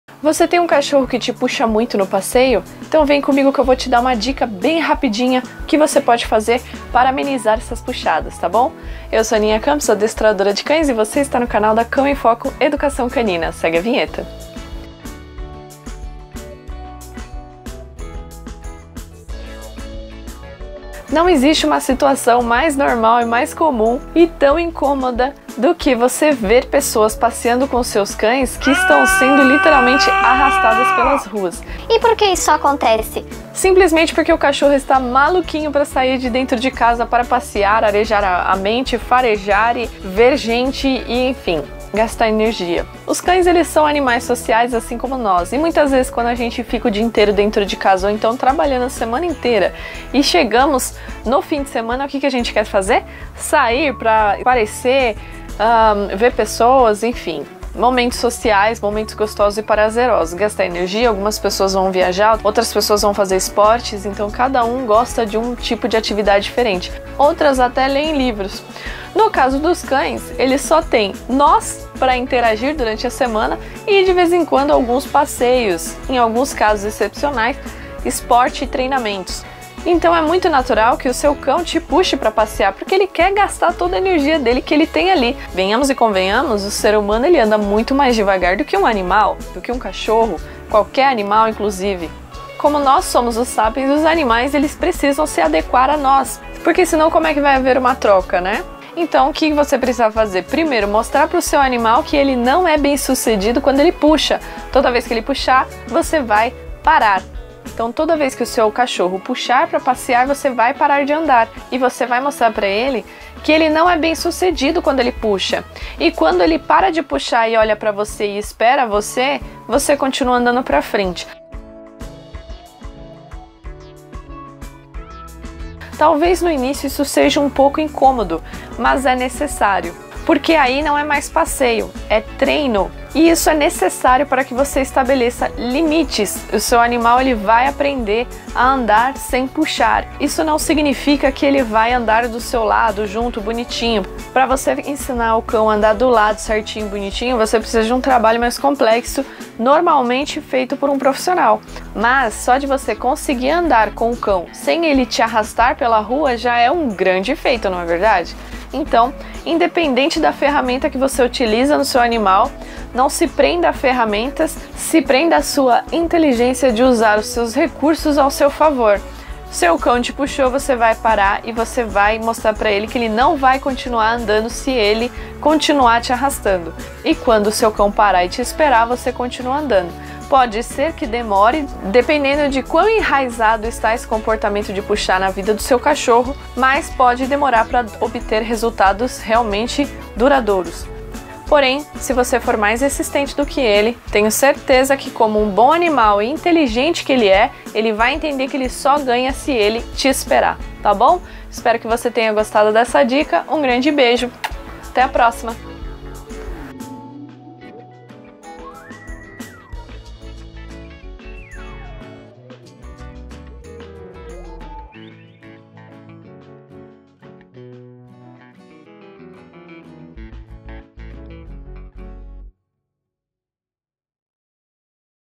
CACHORRO QUE PUXA A GUIA - Dica rápida
Dei uma dica simples para você treinar o seu cachorro a não puxar a guia durante o passeio, te dando mais controle nesse momento, tenha paciência e seja mais teimoso(a) do que ele que você consegue!